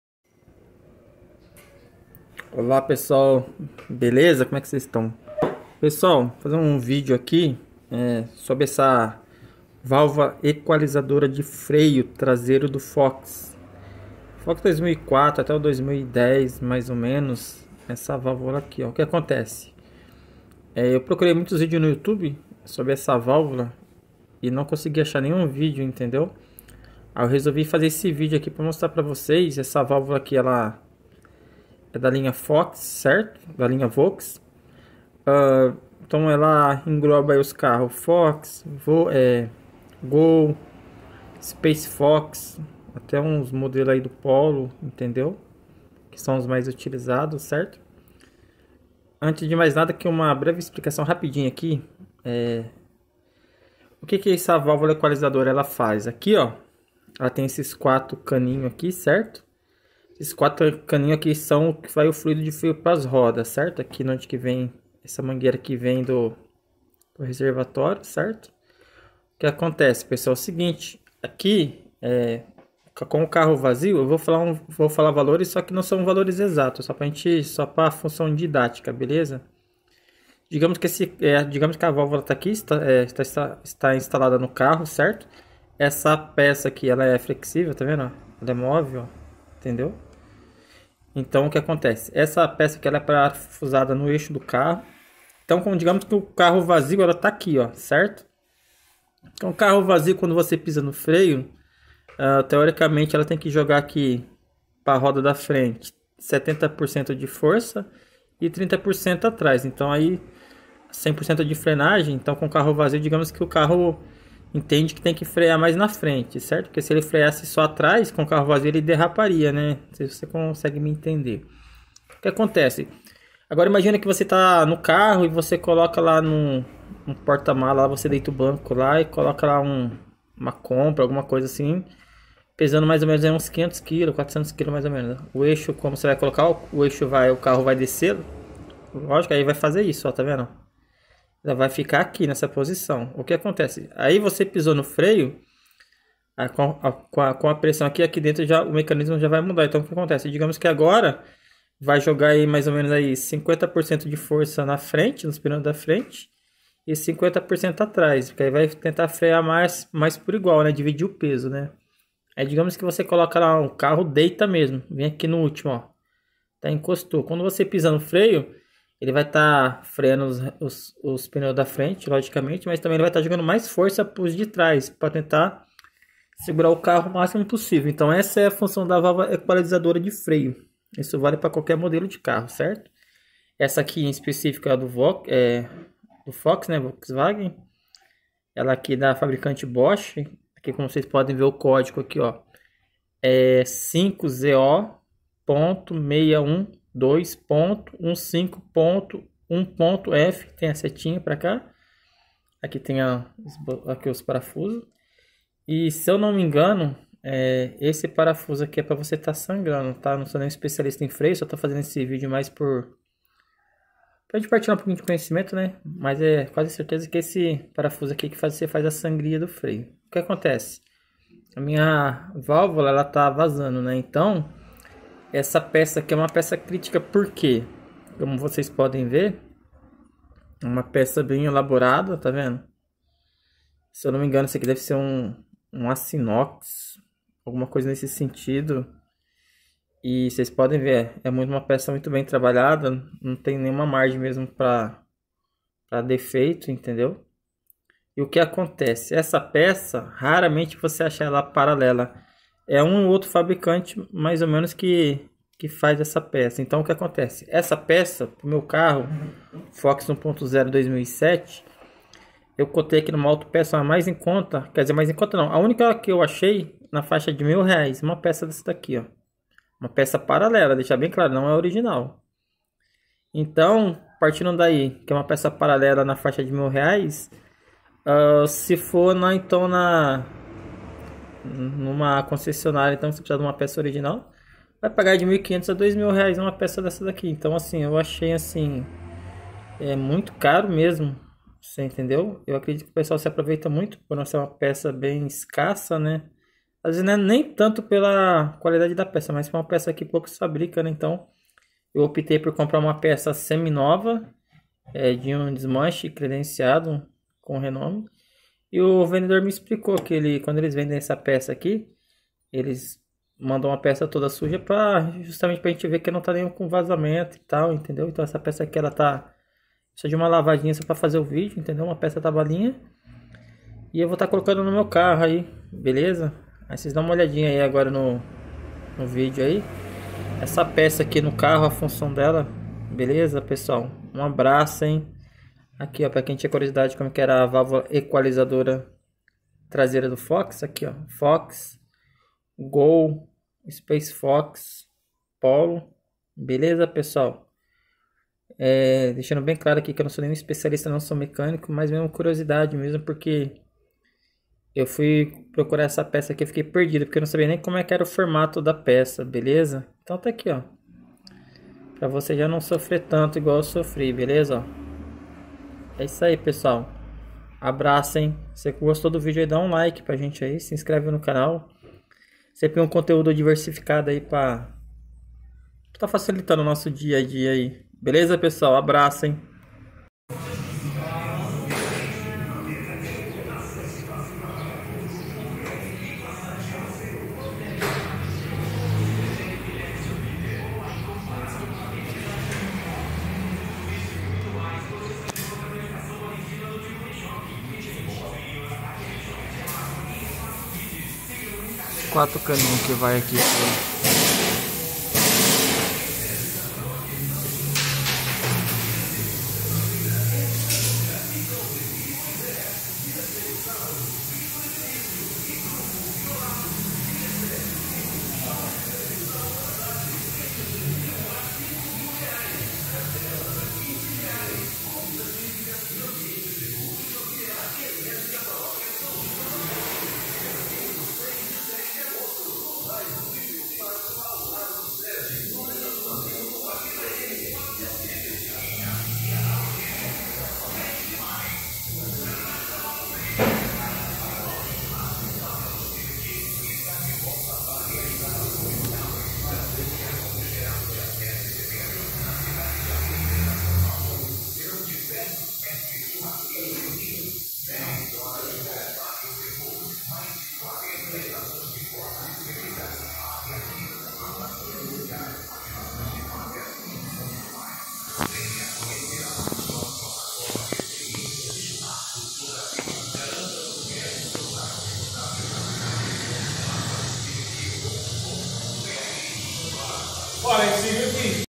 valvula equalizadora freio linha FOX .
válvula equalizadora freio Fox 2004 até 2010, para que serve?